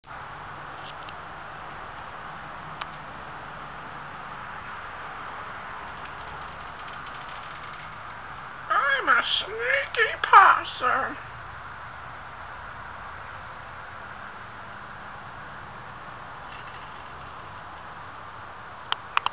Sneaky Possum2
Moved house, but still there is a sneaky possum.  This little ring tail possum has made a nest under my house.